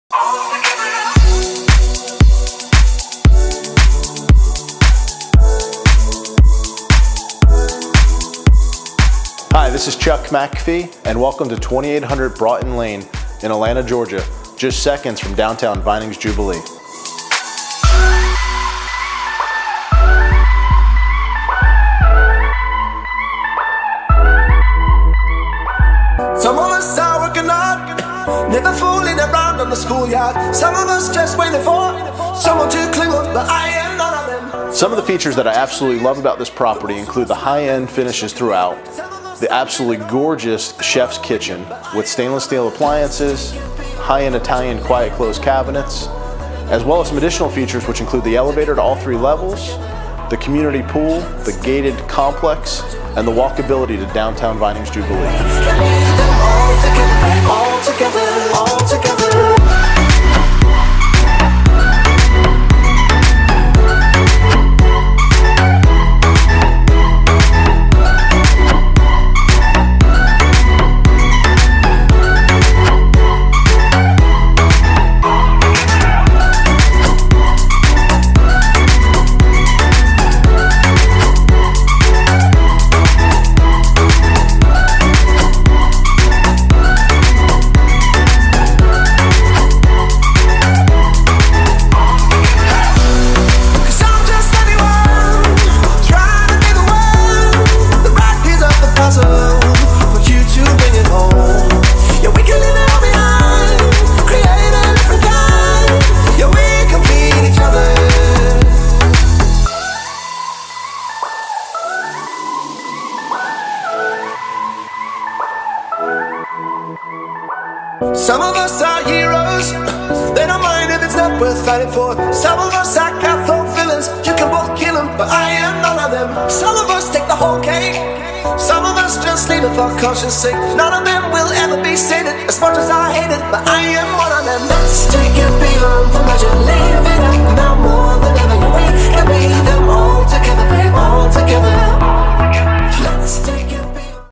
2800 Broughton Ln - Atlanta, GA
Stunning Luxury town home minutes from downtown Vinings Jubilee.